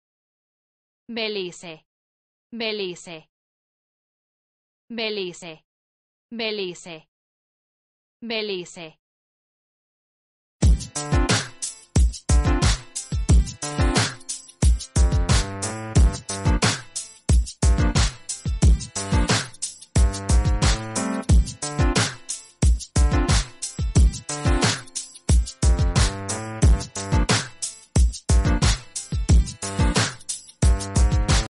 How do you say 'Belize' in Spanish?
How to say 'Belize' in Spanish? Listen to hear the pronunciation.